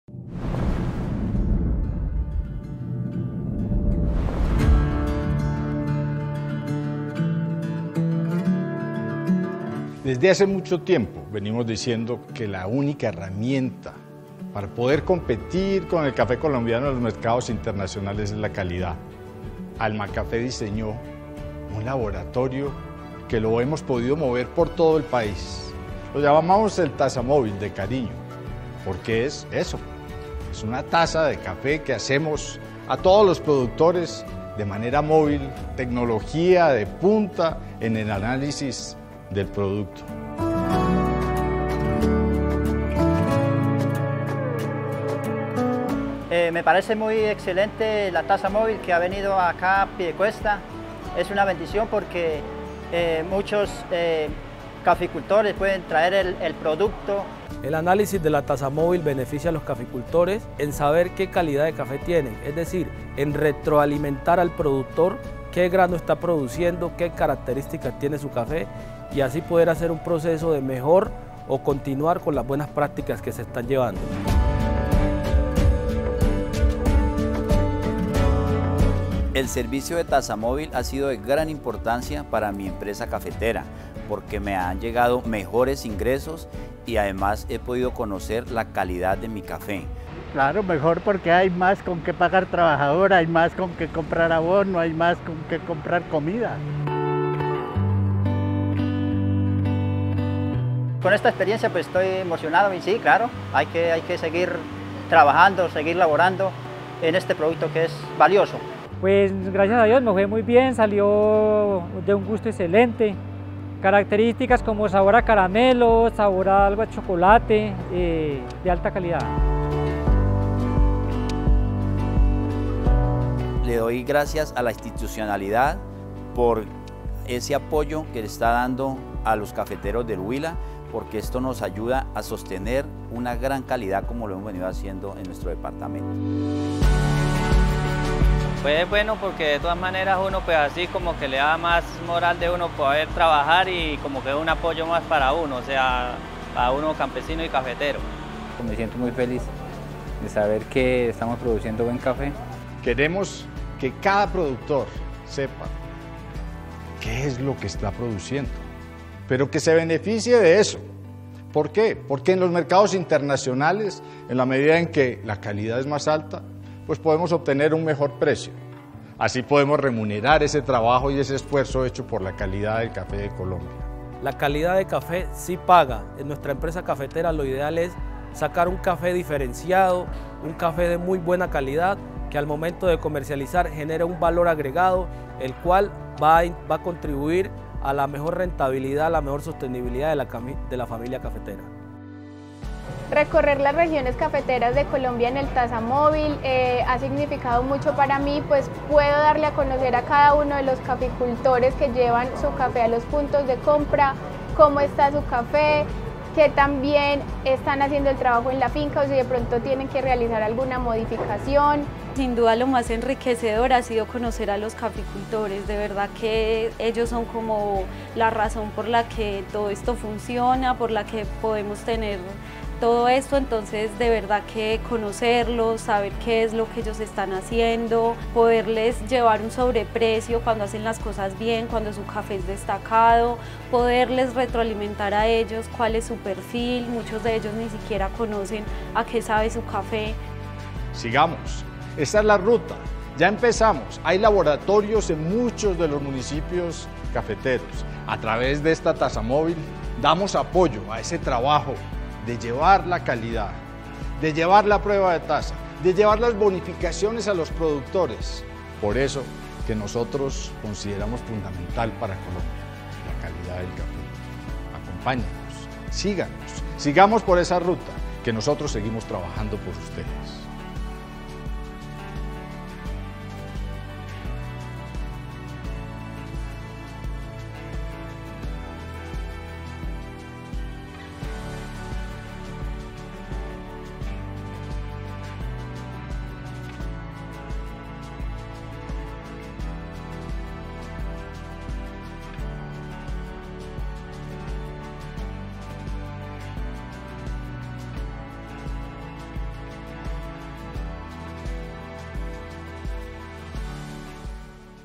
Taza Móvil, nueva estrategia de FNC para llegar a todas las regiones y valorar la calidad del café
Con un vehículo acondicionado como laboratorio, la FNC, de la mano de su brazo logístico Almacafé, llega a algunos de los rincones más apartados del país para valorar mejor la calidad de su café.